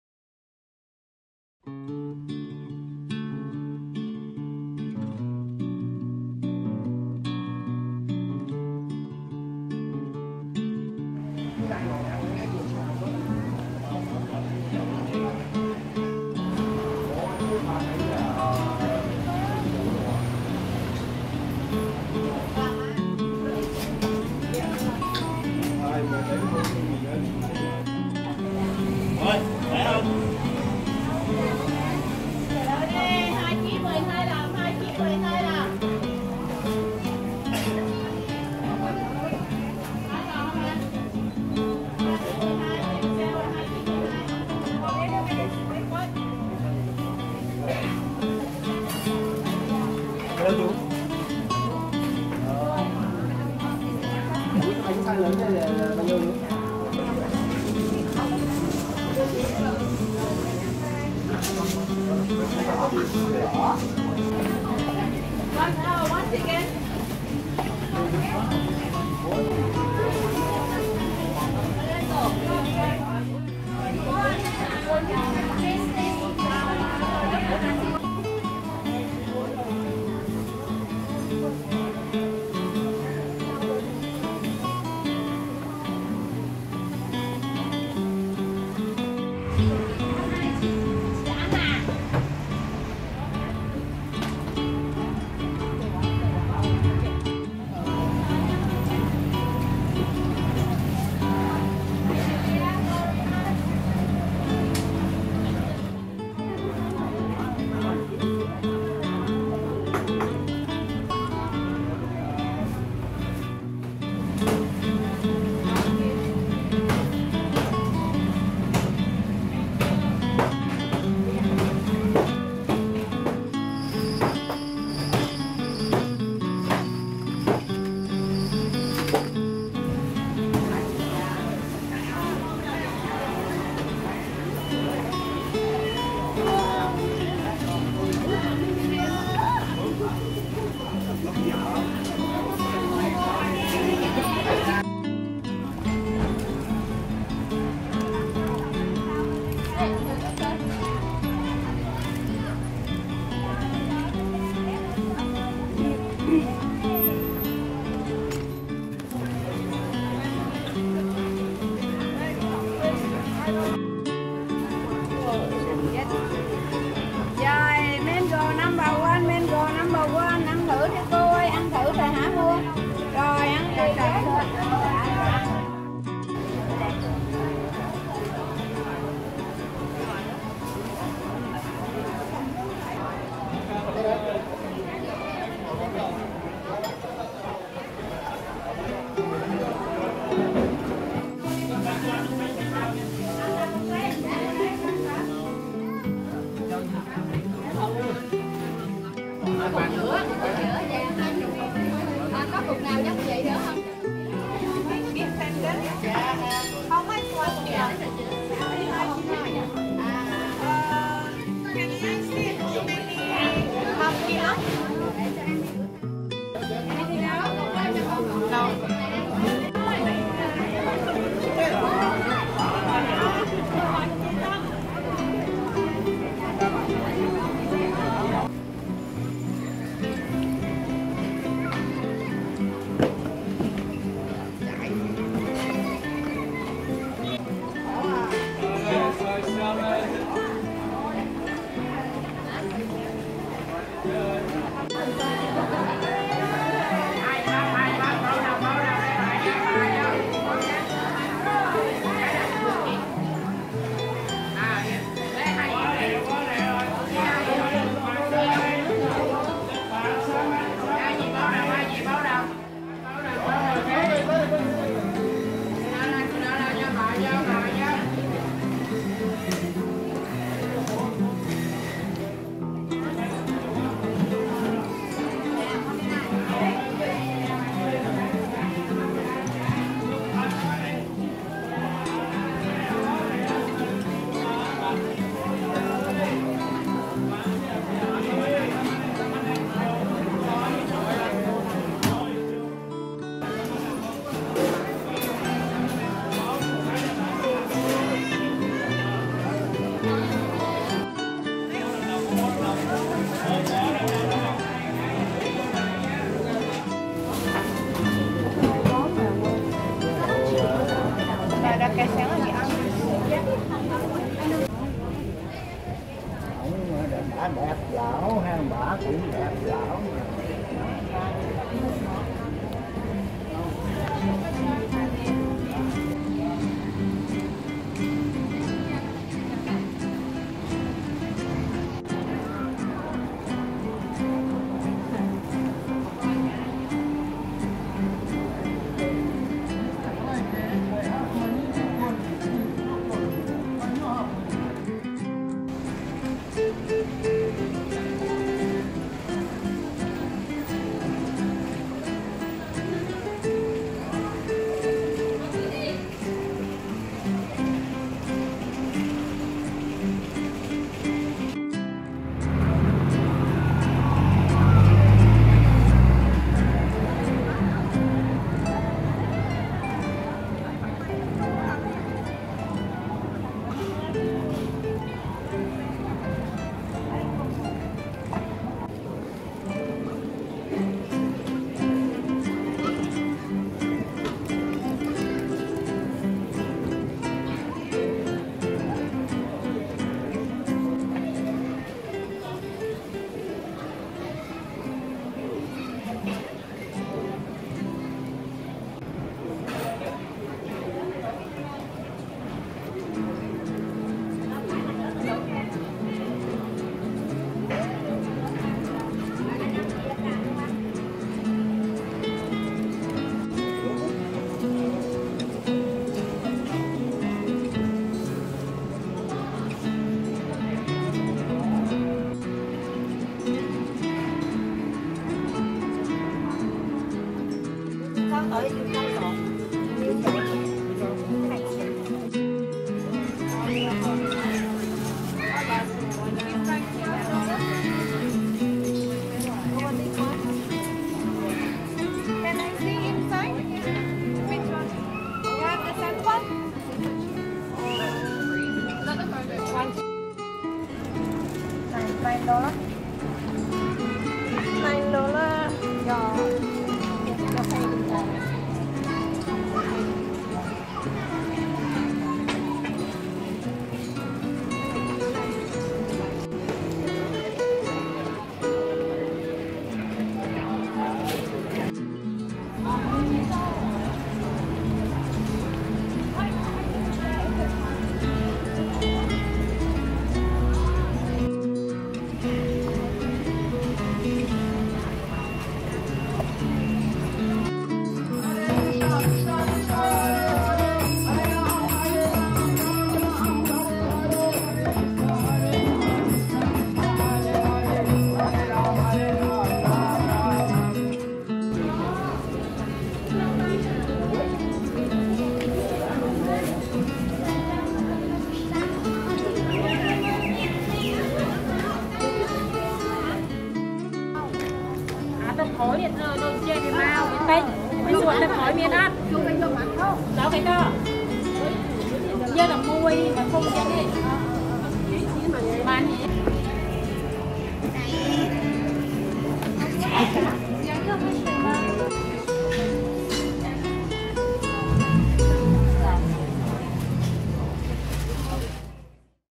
Weekend Notes: Oct 2017 Groceries At Cabramatta
Relaxing weekend, accompanying my wife to shop groceries at Cabramatta. A suburb in south western Sydney. It has the largest Vietnamese community in Australia.